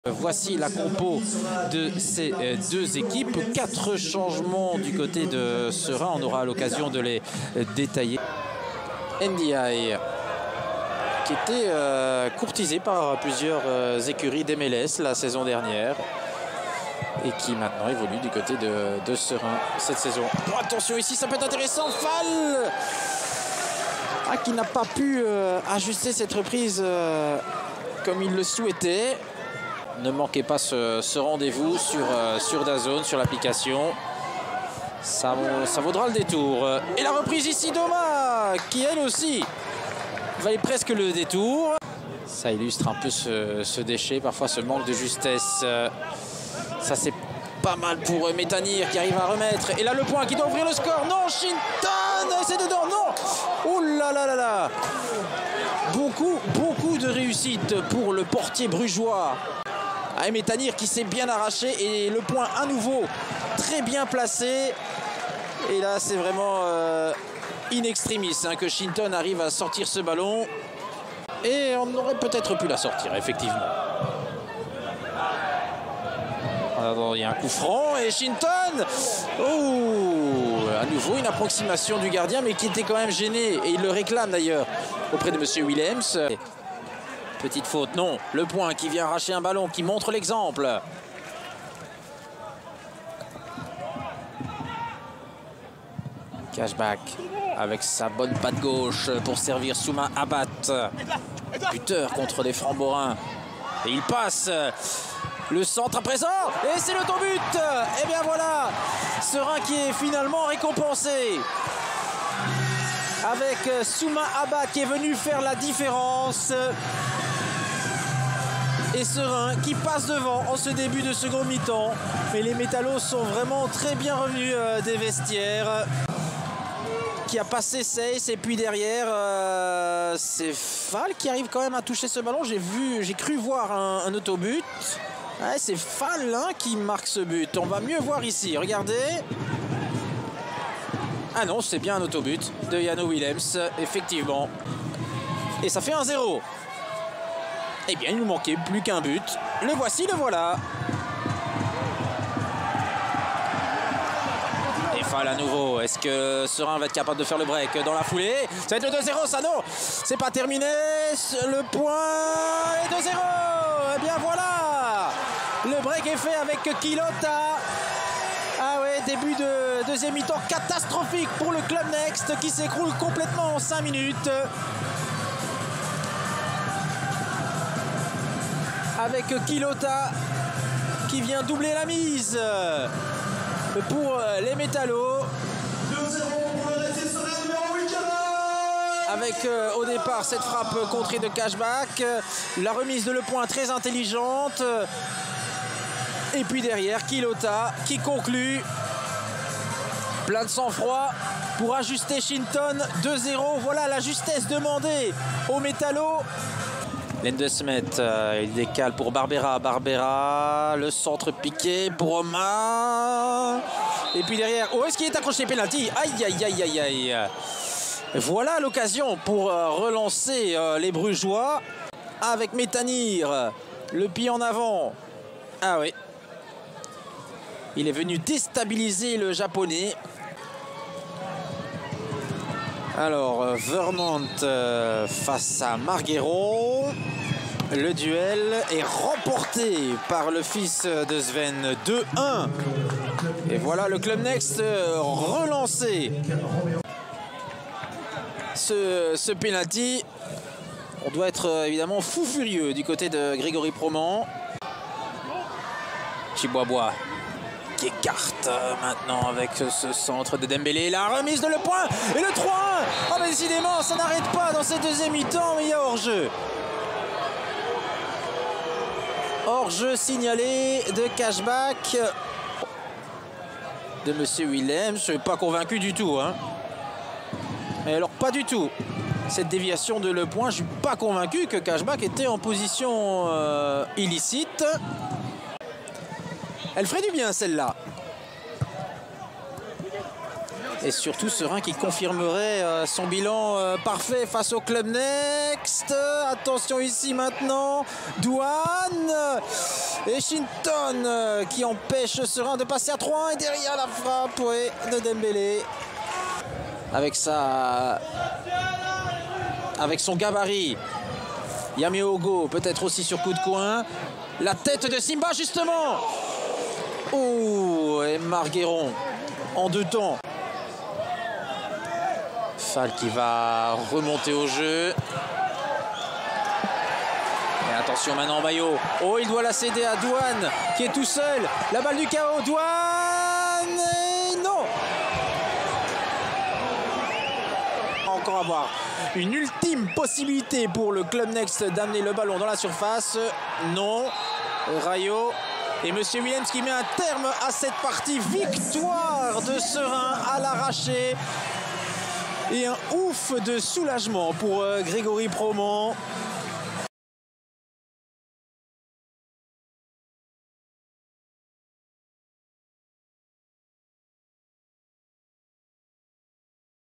RÉSUMÉ : 🔴⚫ RFC Seraing vs Club NXT 🔵⚫ | 28.10.2023 | 2-1
10e match de la saison 2023/2024 - D1B - Challenger Pro League.

✅ Victoire face au Club NXT | 2-1 | Stade du Pairay.